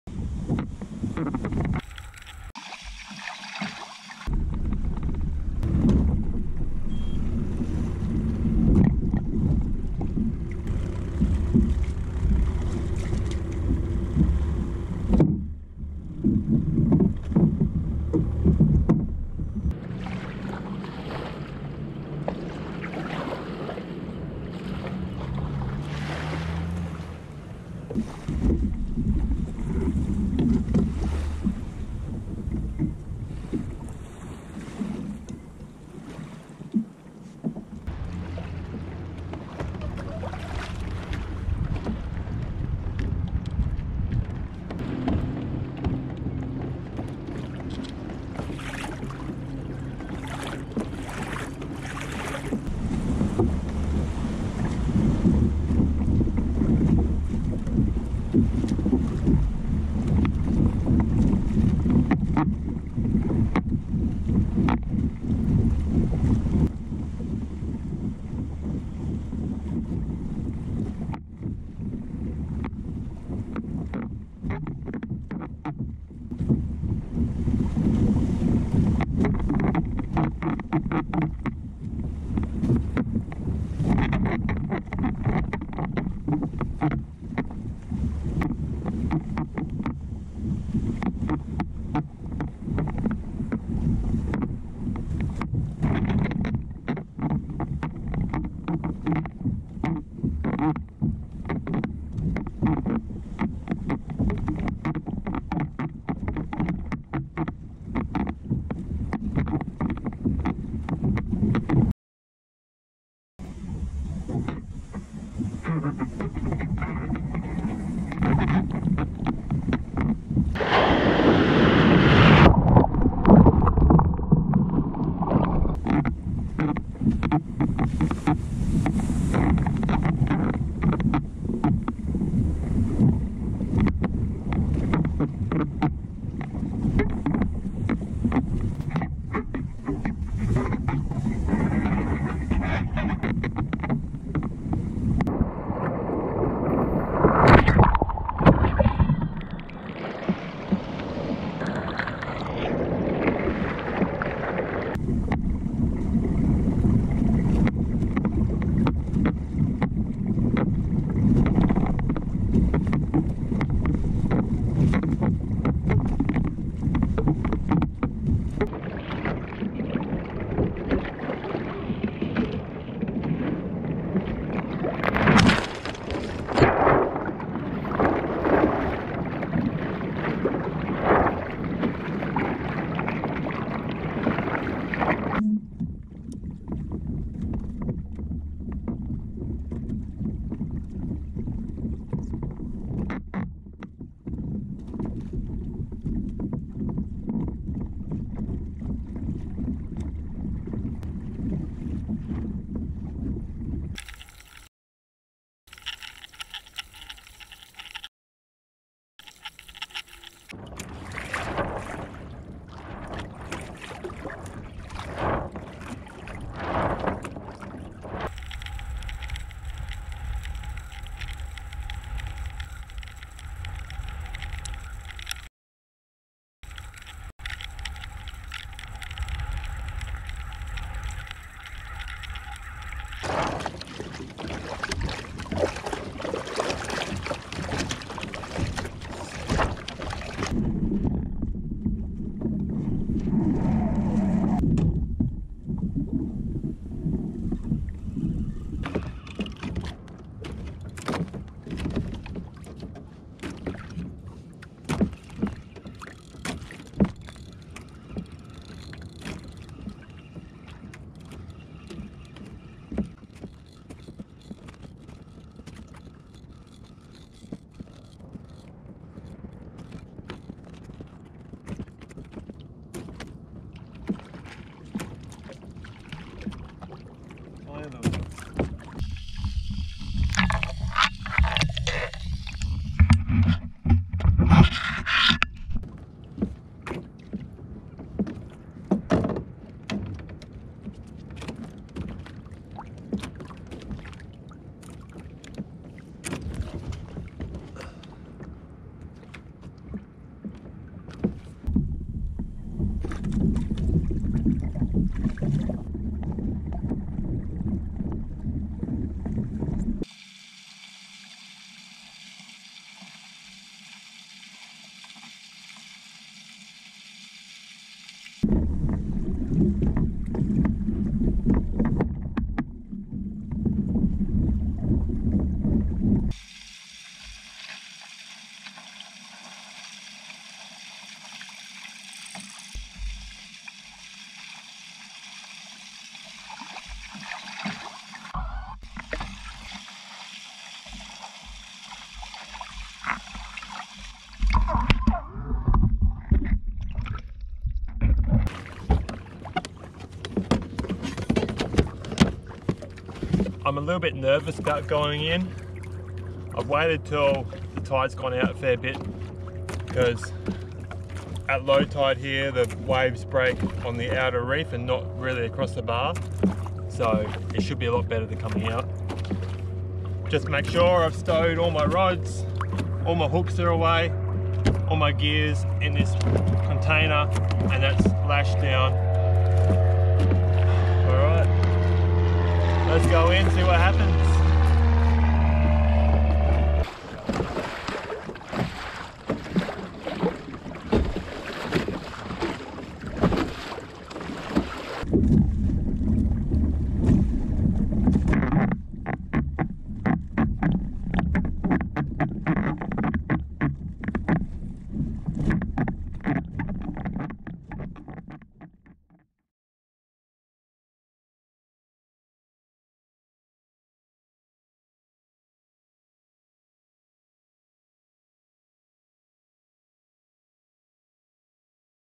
Hobie Tandem Adventure Island bar crossing and fishing //Yellowtail Kingfish //Tuna // Mackerel
Scary bar crossing turned into a very calm day on the reef.
Still learning how to handle the Tandem Adventure Island, but she took on some decent waves like a champion!
Tried out some new camera angles.
Some fish showed up on the troll camera, but weren't interested in my offerings...except for one undersized Mackerel.